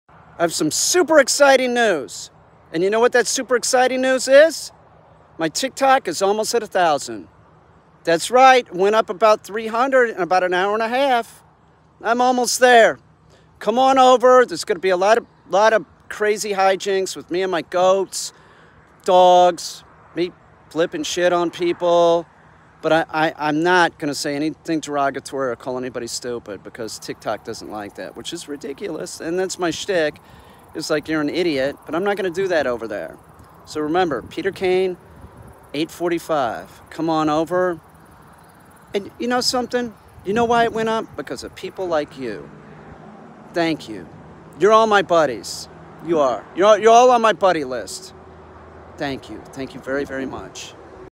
Super Exciting News
These are my goats they live inside, for now